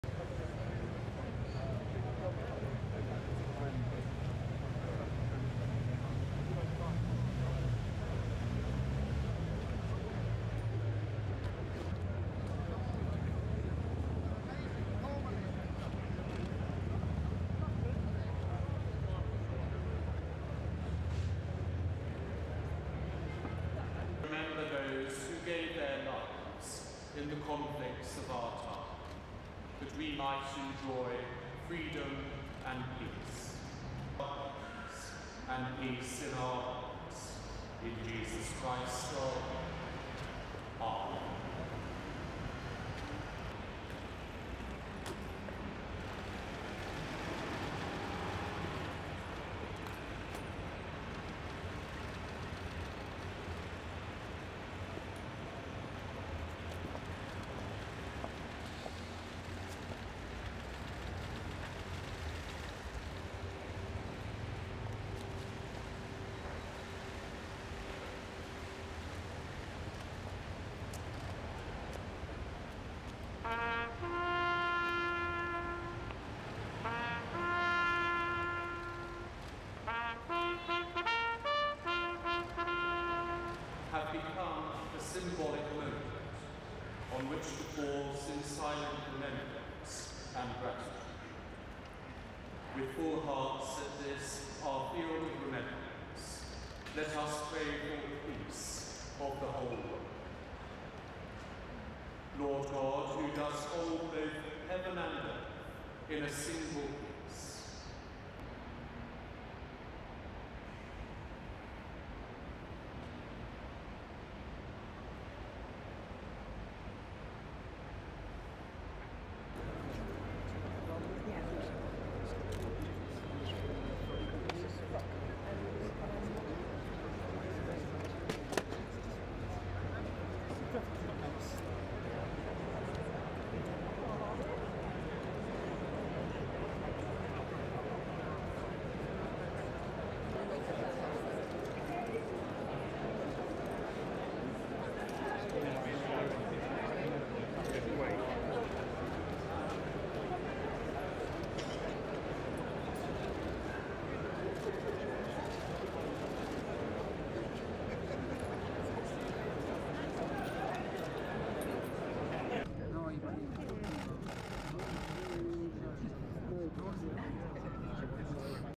Harry, Meghan honour war dead at Westminster Abbey
(7 Nov 2019) The Duke and Duchess of Sussex on Thursday attended a ceremony at Westminster Abbey honouring the country's war dead in different conflicts in the world.
The royal couple laid two miniature crosses on the Field of Remembrance before observing a two-minute silence.
They toured the grounds of the Abbey where members from different regiments, military associations and other organisations waited to greet them.
The Duchess of Cornwall did not attend the event due to ill health.